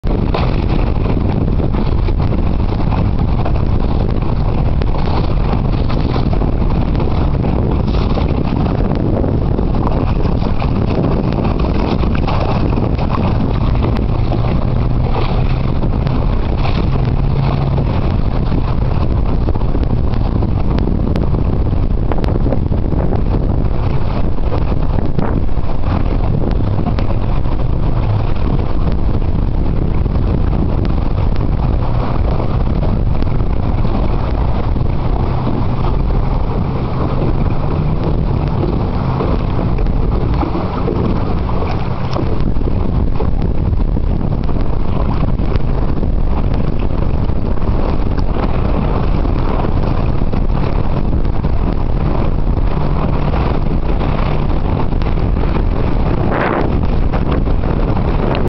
Michalak Blobster Trial Sail.AVI
The first sail of 'Bob' Blobster on Lake Macquarie NSW Australia on 8 Jan 2010. She sailed beautifully and I was really delighted with her performance. Note that the G clamps at the mast partner are being used to determine the best balance for the CE and CLR. Raking the mast aft by 1/4 of an inch gave just a little weather helm - perfect!. Jim suggests playing around with this to get the right balance for your boat. Every boat will be just a little different in this respect so it is worth fiddling with.